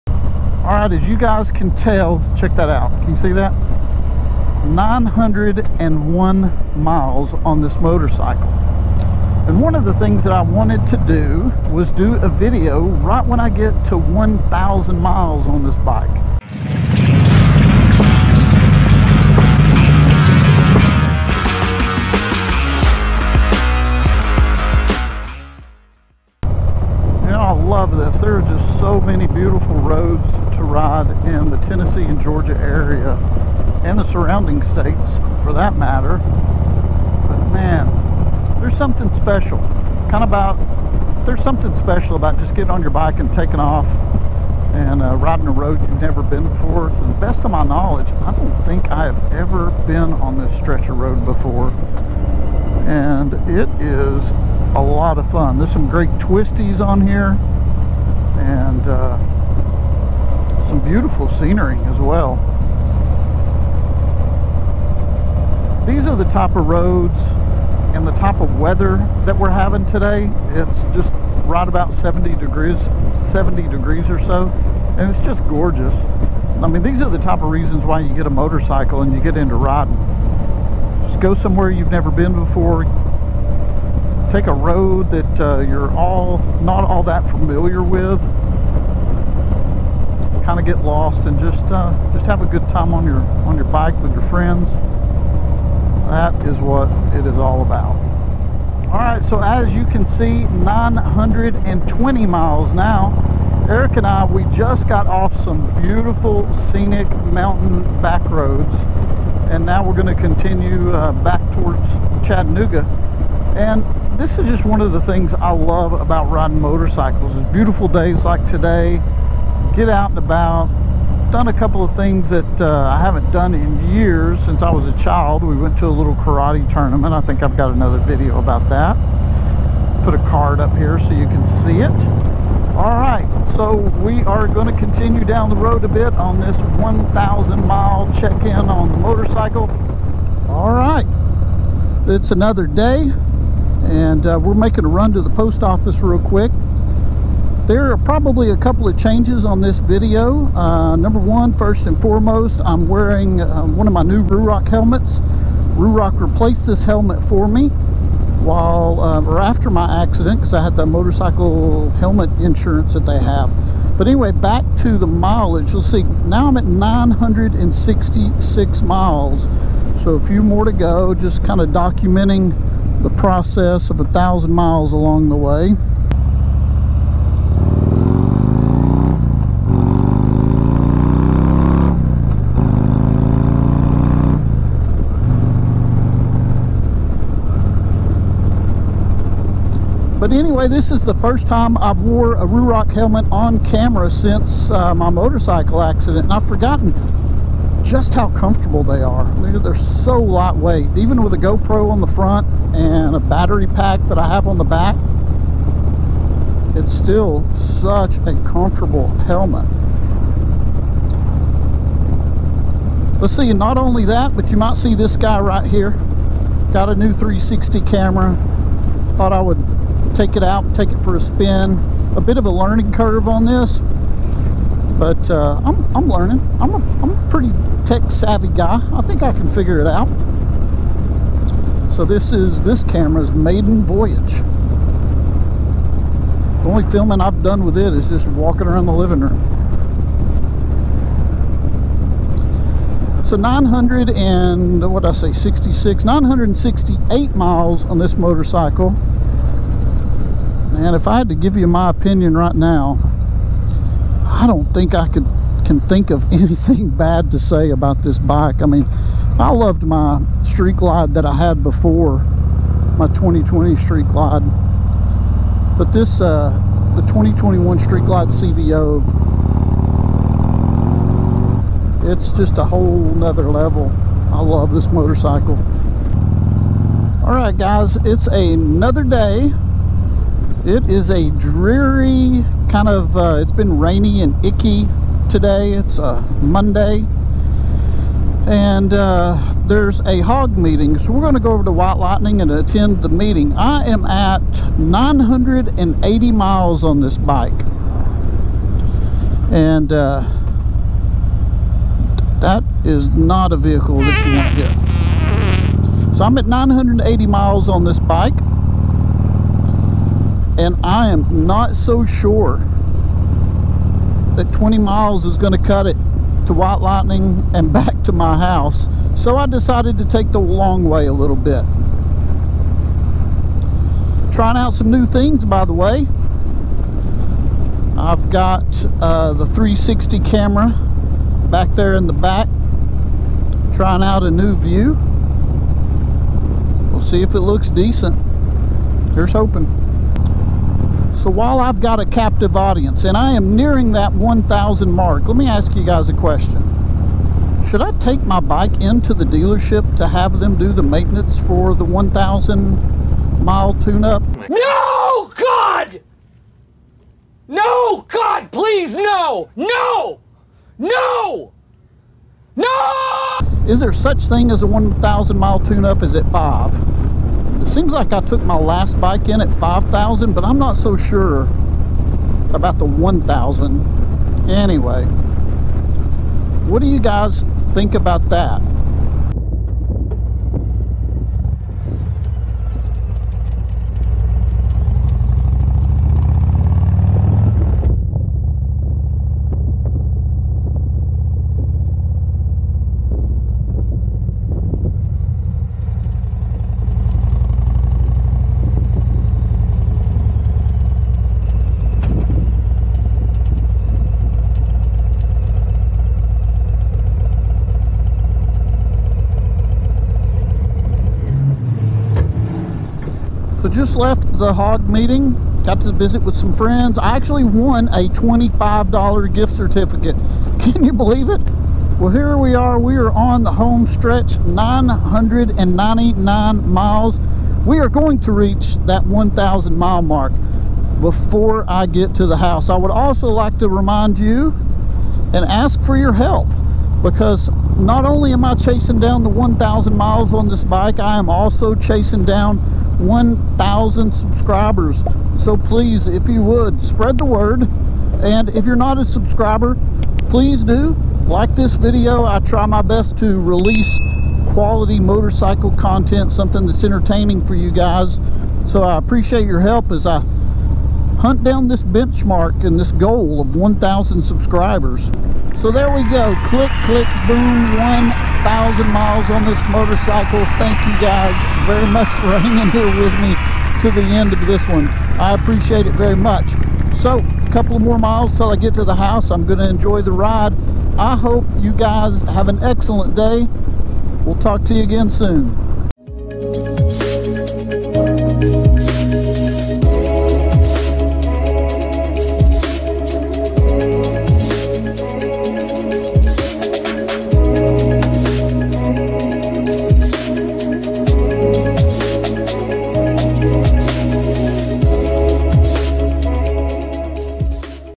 2021 Harley Davidson Street Glide CVO | First 1000 Miles
Please join me as I review my first 1000 miles on my 2021 Harley Davidson Street Glide CVO. The past 1000 miles on this motorcycle have been so much fun!. I also attempt to make some 360 camera edits...

⏱️TIMESTAMPS⏱️
0:00​​​​​​ - Intro
0:14 - Day 1
2:20 - Day 2
4:35 - Day 3

I would like to trade stickers, poker chips, etc with other motovloggers!
Ty From Tennessee
P.O.Box 2362